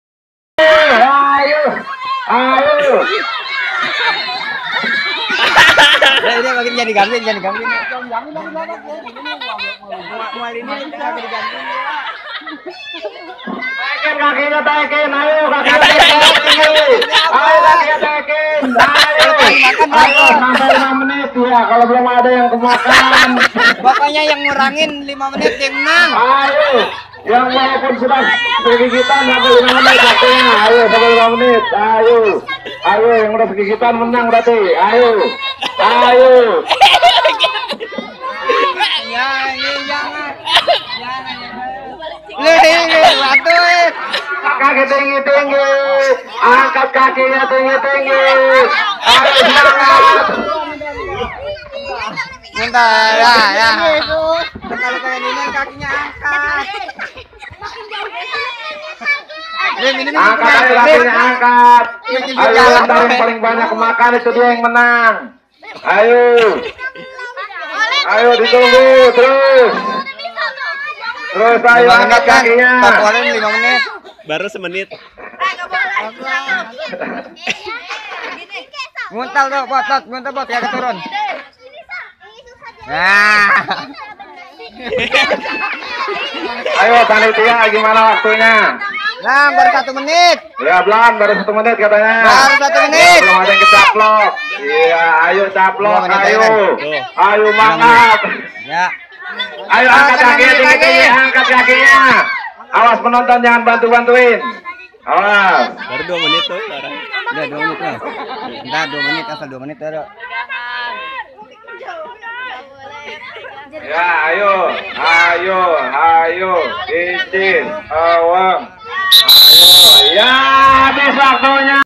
Lomba makan krupuk pake tali. Dalam rangka Hut RI
Lomba makan kerupuk di kampung karet tajurhalang, kabupaten Bogor.